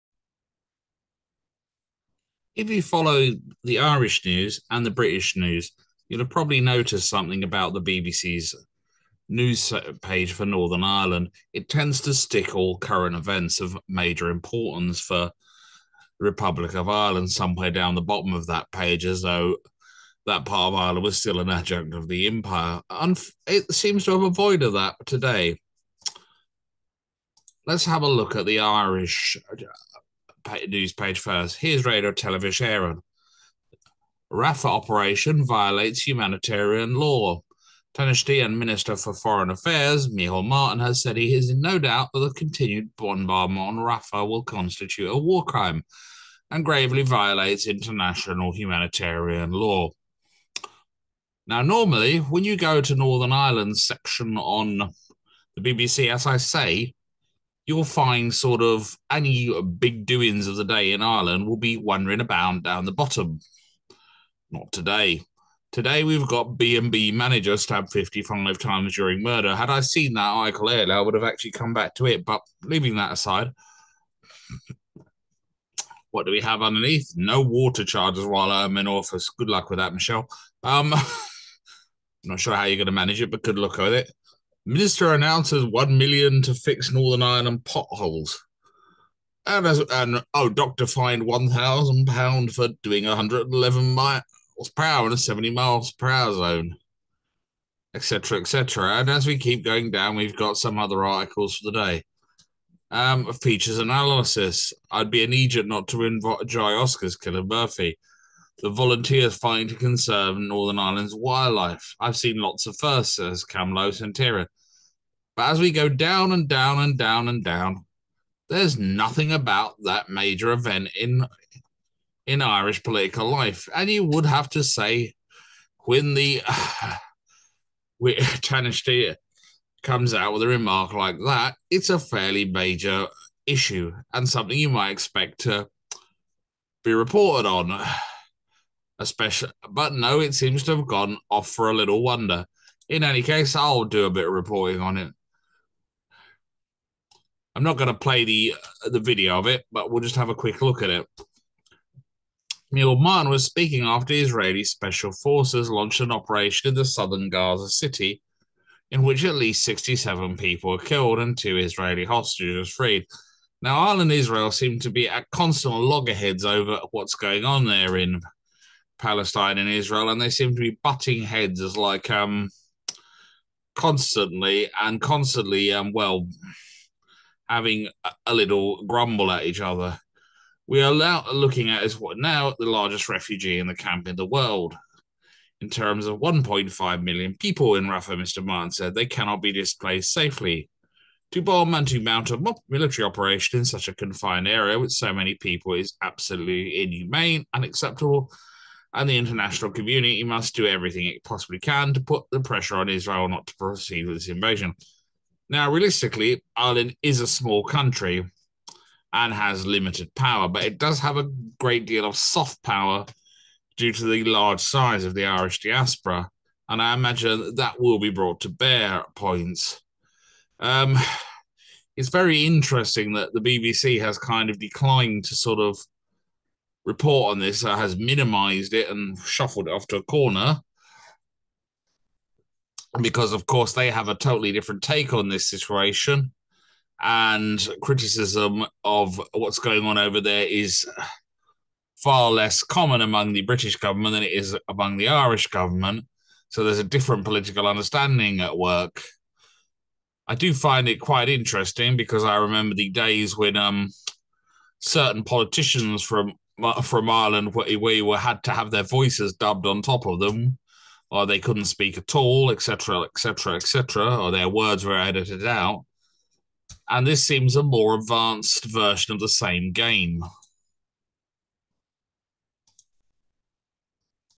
Selective Reporting - How Two State Broadcasters Are Reporting on Israel.
RTE is the Irish equivalent of the BBC in many ways. The news that the Tánaiste had been highly critical of Israel and the situation in Gaza was big news on that broadcaster today.

Normally, when you go and look at the news for the day about Northern Ireland on the BBC's site they tend to stick all major happenings for the Republic of Ireland down the bottom of the page. Strangely, they seem to have forgotten all about these comments by a senior member of the Republic of Ireland's government today.